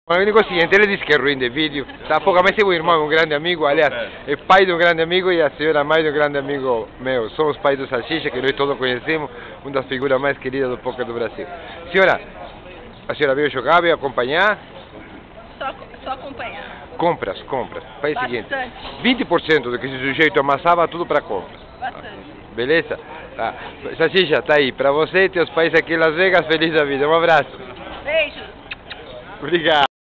Familia Salsicha
Tevez encontra os Pais do jogador Brasileiro "Salsicha"